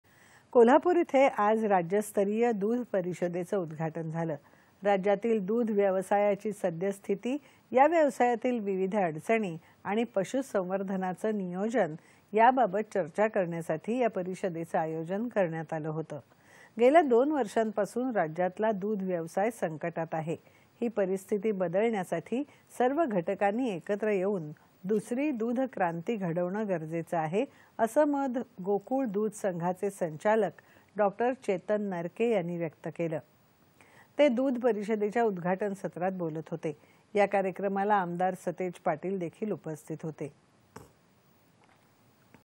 कोल्हापूर इथं राज्यस्तरीय दूध परिषदेचं उदघाटन
दि २०.०१.२०२३ :
 कोल्हापूर इथं आज राज्यस्तरीय दूध परिषदेचं उदघाटन झालं. राज्यातली दूध व्यवसायाची सद्य स्थिती, या व्यवसायातील विविध अडचणी आणि पशुसंवर्धनाचं नियोजन याबाबत चर्चा करण्यासाठी या परिषदेचं आयोजन करण्यात आलं होत.  गेल्या २ वर्षापासून राज्यातला दूध व्यवसाय संकटात आहे.  ही  परिस्थिती बदलण्यासाठी सर्व घटकांनी एकत्र येऊन दुसरी दूध क्रांती घडवण गरजेचं आहे, असं मत गोकुळ दूध संघाचे संचालक डॉ चेतन नरके यांनी व्यक्त केलं. ते दूध परिषदेच्या उदघाटन सत्रात बोलत होते. या कार्यक्रमाला आमदार सतेज पाटील देखील उपस्थित होते.